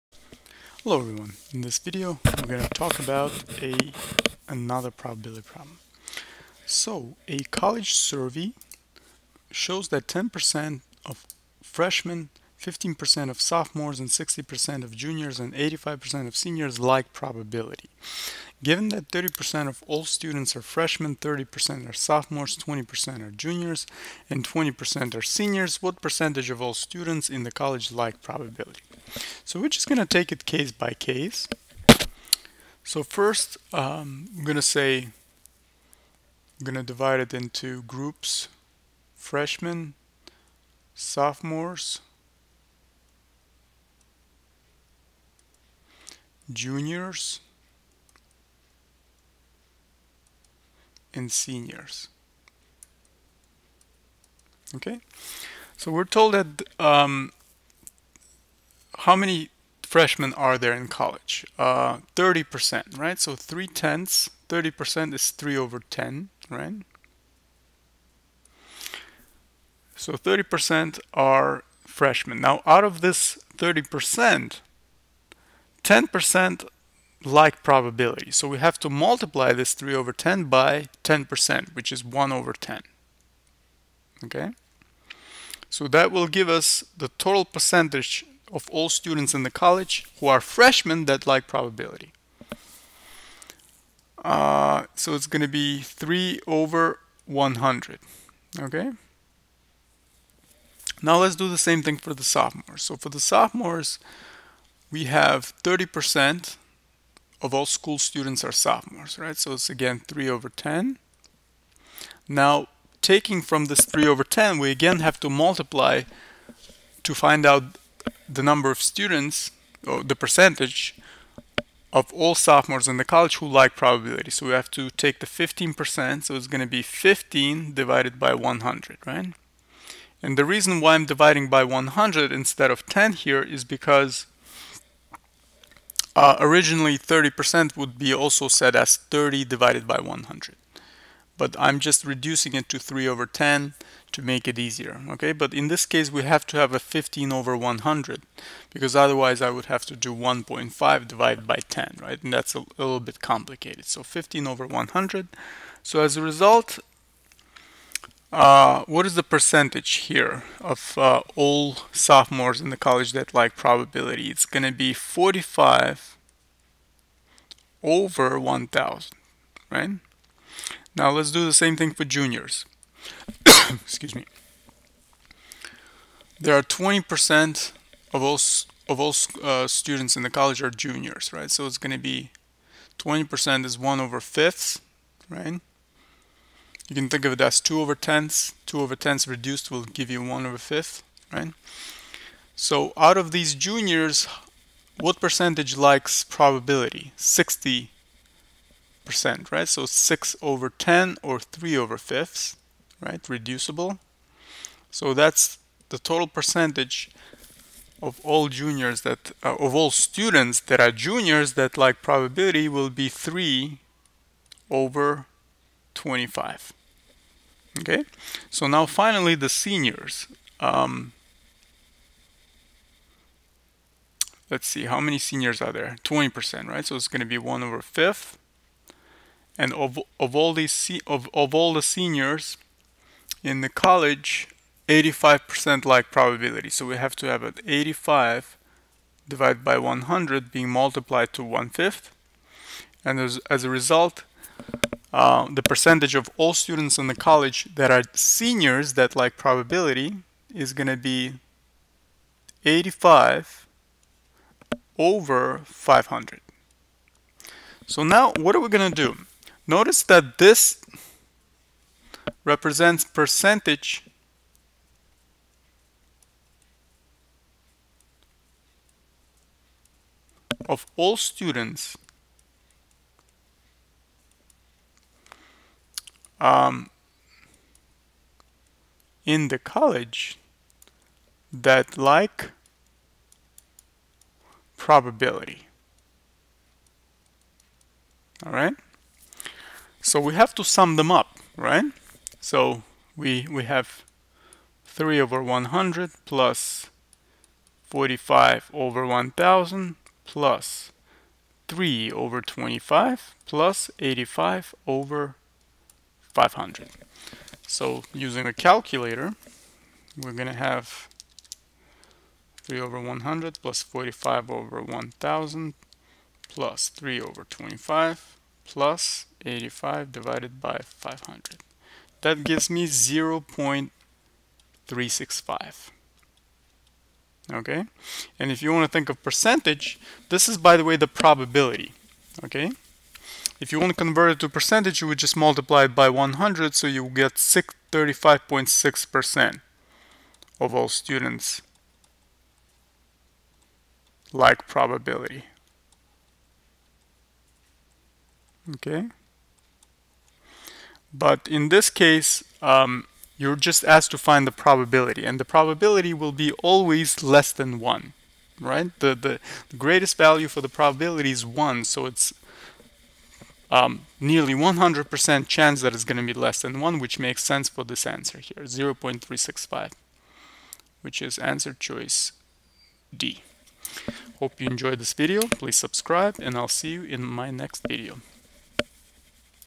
🎲Percent Probability problem ! ! ! ! !
In this video you will learn how to relate given Percentages with Probability.

A college survey shows that 10% of freshmen, 15% of sophomores, 60% of juniors, and 85% of seniors like Probability. Given that 30% of all students are freshmen, 30% are sophomores, 20% are juniors, and 20% are seniors, what percentage of all students in the college like Probability?

0:28 Writing fractions and products from given percentages —
 finding fractions for each group of students that like Probability
4:49 Adding all found fractions (groups of students) to find the overall Percentage of students that like Probability

Bayes' Formula: Conditional Probability problem #1

Bayes' Formula: Conditional Probability problem #2

Bayes' Formula with Venn Diagram

Probability: Combination problem (using nCr)